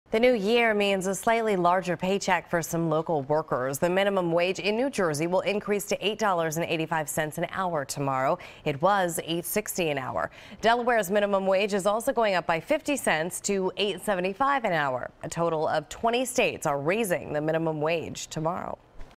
New Jersey, Delaware Minimum Wage Hikes Go Into Effect Tuesday
20 states have raised their minimum wage in 2019.